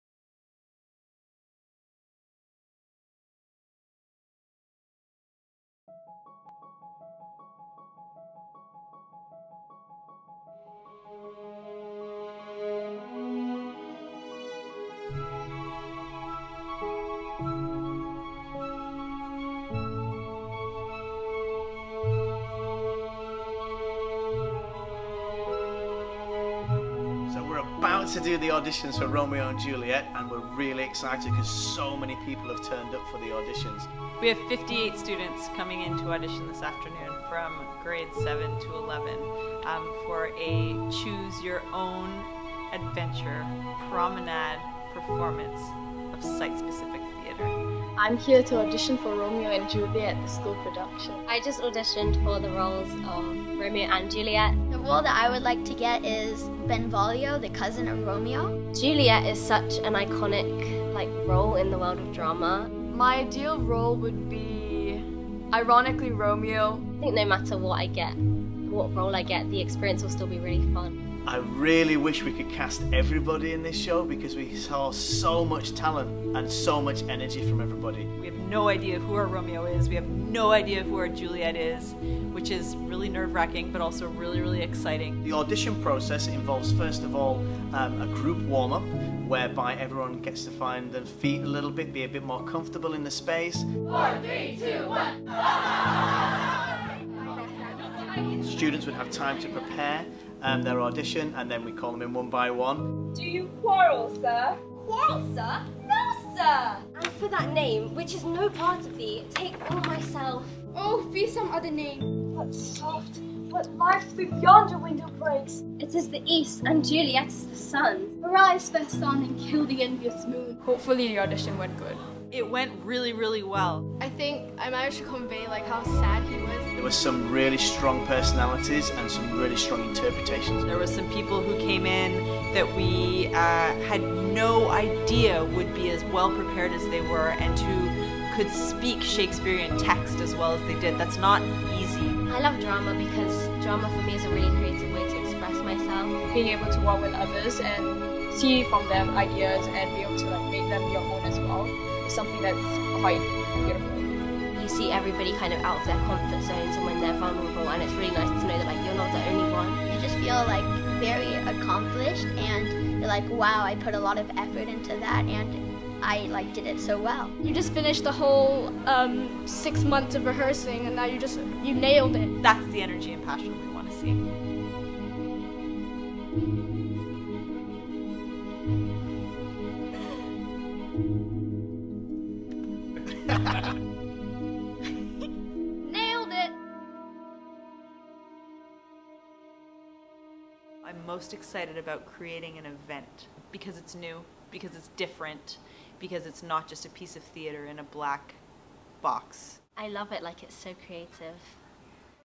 SJI International Romeo & Juliet The Auditions Process
In 2019 our Drama Department presented a modern take of the classic tale Romeo and Juliet, in partnership with the Purple Community Fund to raise funds for the households in Tondo, Manila.